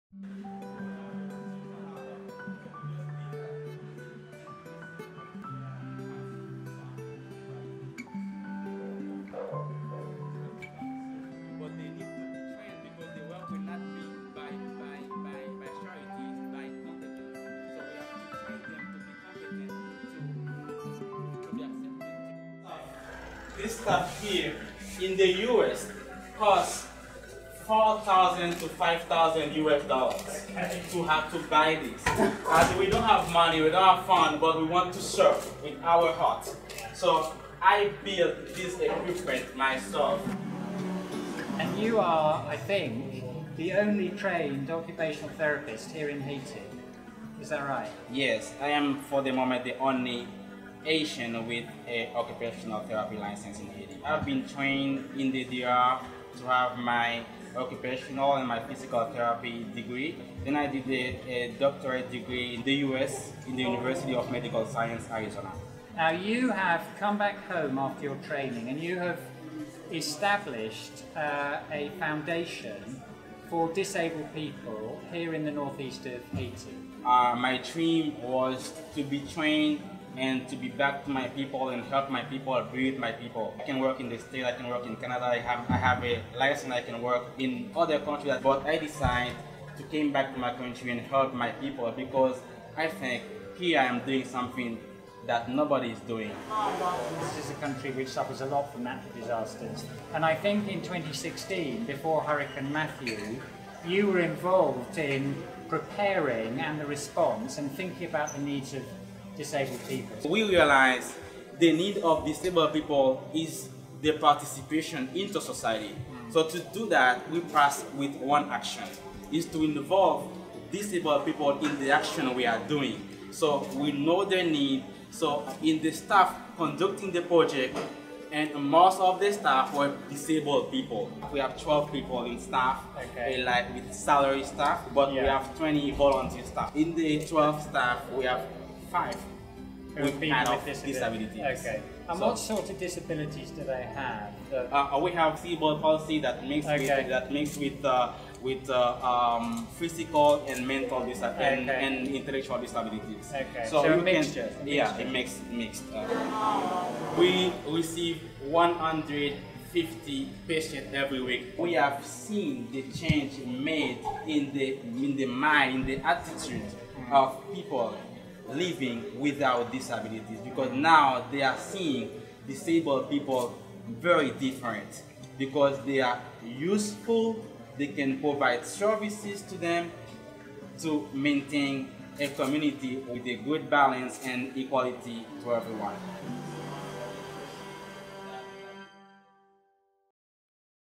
FONHARE - Interview with Ivens Loious in Haiti
The Fondation Haitienne de Réhabilitation
(FONHARE) provides physical and cognitive
rehabilitation services to individuals with
physical and/or mental disabilities, regardless of their economic capacity.